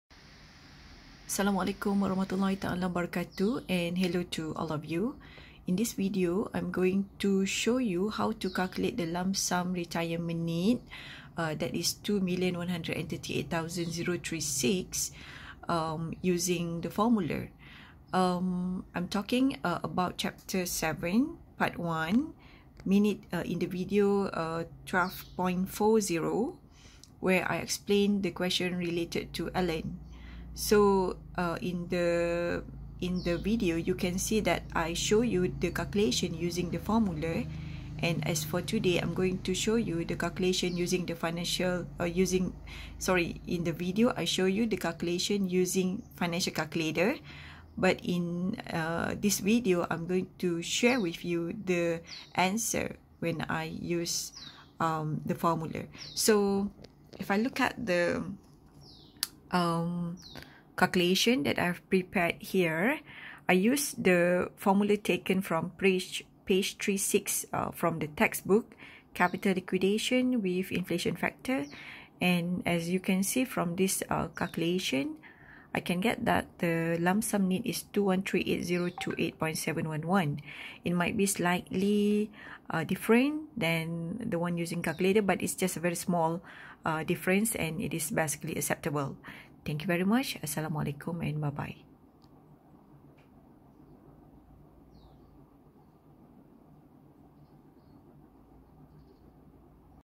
Chapter 7: Construction of Retirement Portfolio II Retirement Planning II (Part 3)
(How to calculate lump sum need using FORMULA)